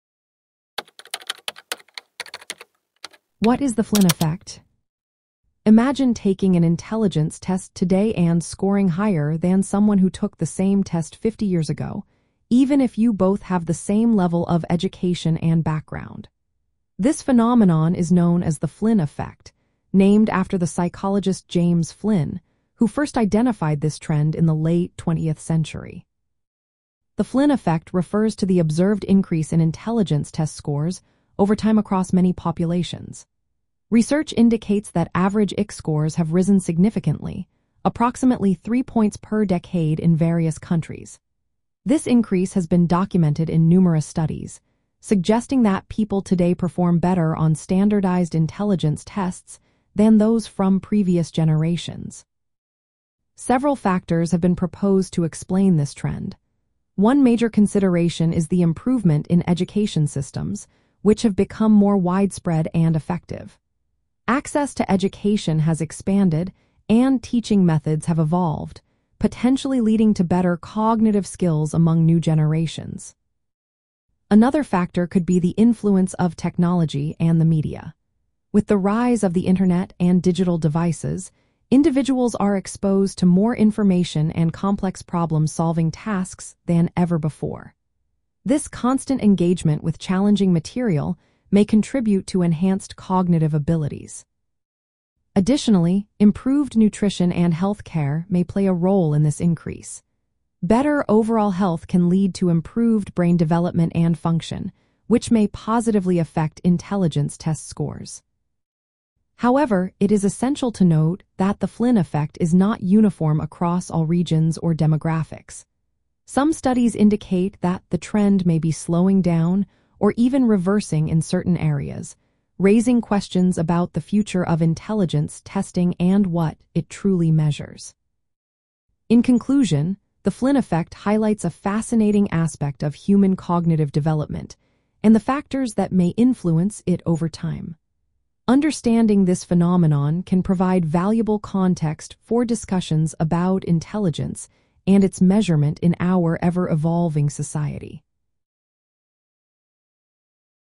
What Is The Flynn Effect? - The Friendly Statistician
What Is The Flynn Effect? Have you ever heard of the Flynn Effect and its implications on intelligence testing? In this engaging video, we will unpack the concept of the Flynn Effect, a term that describes the trend of rising intelligence test scores over time. We will explore how average IQ scores have changed across different populations and what factors may contribute to this phenomenon.

Throughout the video, we will discuss the role of education systems, advancements in technology, and improvements in nutrition and healthcare in shaping cognitive abilities. Additionally, we will examine the varying impact of the Flynn Effect across different regions and demographics, including the possibility of a slowdown or reversal in some areas.

By understanding the Flynn Effect, you can gain a broader perspective on intelligence and its measurement in our society. This discussion is not only relevant for those interested in psychology and education but also for anyone curious about how our world evolves and influences cognitive performance.